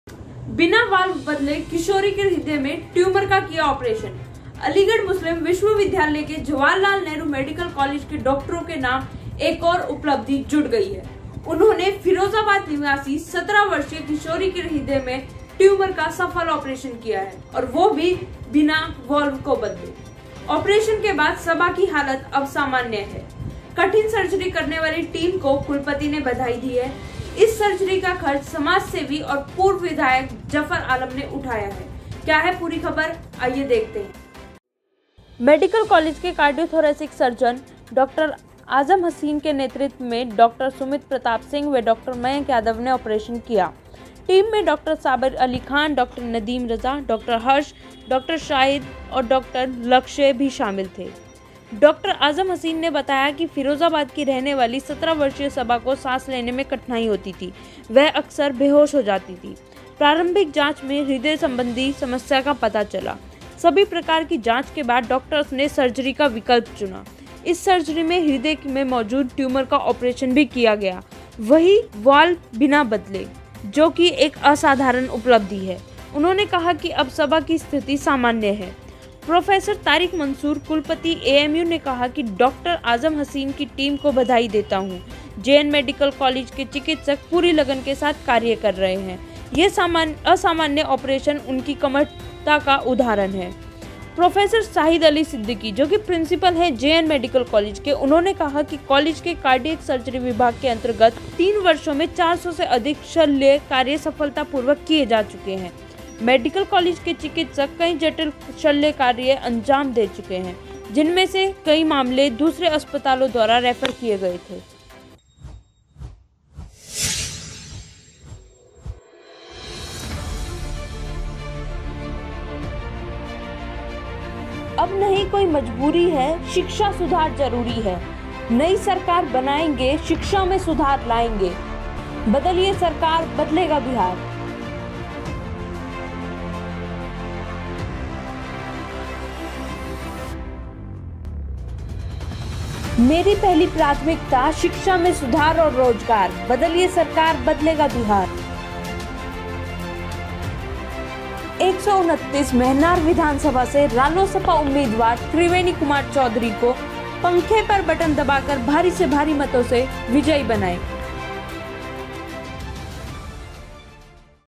अलीगढ़: वाल्व बिना बदले किशोरी के हृदय में ट्यूमर का किया आपरेशन
Aligarh#amu#uttar pradesh